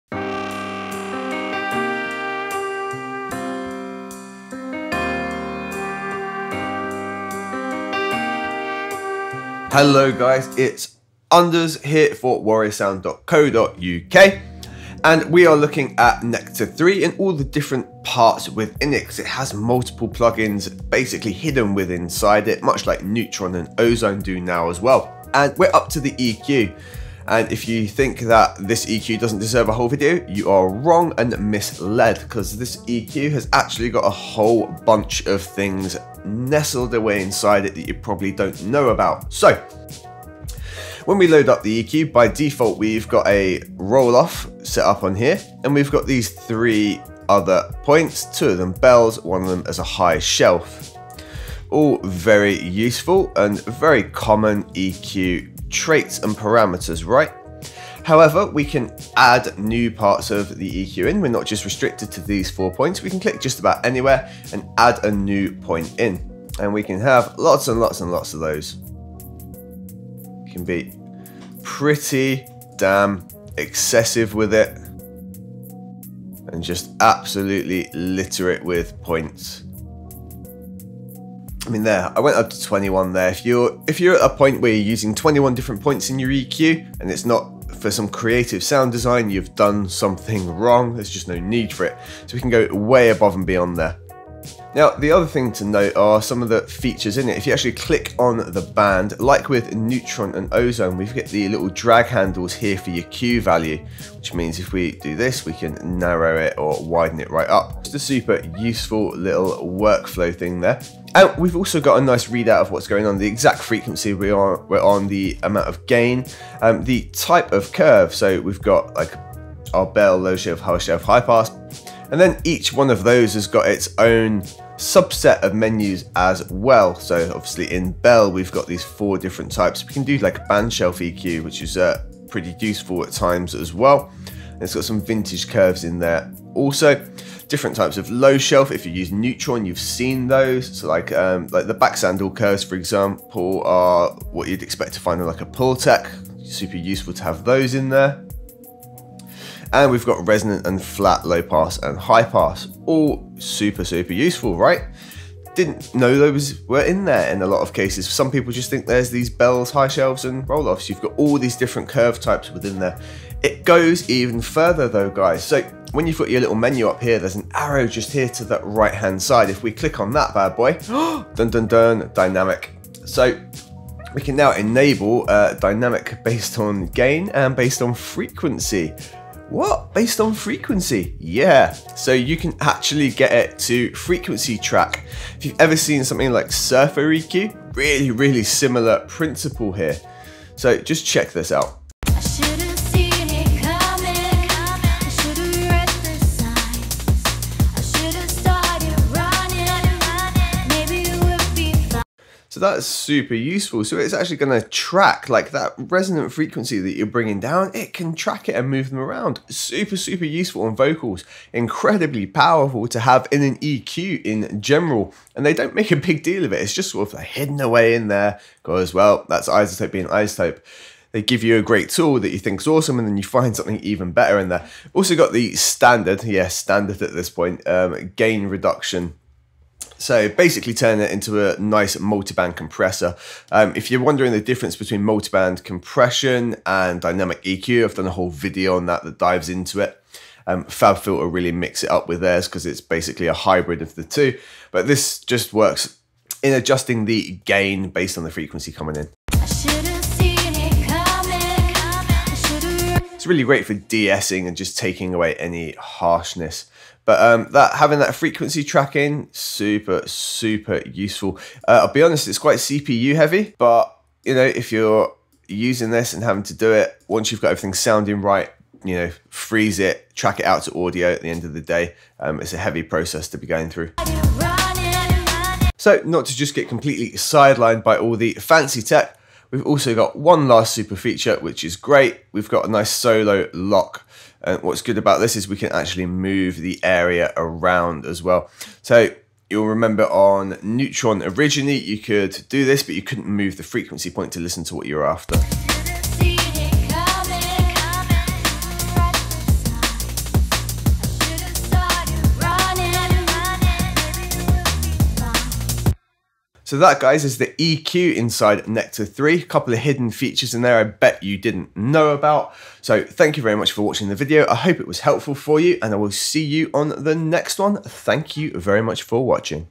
Izotope Nectar 3 EQ Hidden Features
Dynamic EQ
Dynamic EQ mode responds to the dynamic level of your audio, allowing you to make transparent cuts or boosts.

Follow EQ
Follow EQ Mode will ride selected frequencies and their harmonics for unparalleled control of pitch information so you don’t have to make hours of corrective manual edits by hand.

Releasing your music and want to make sure your fans stream your music? I put together a free guide of what i'm doing,
USEFUL STUFF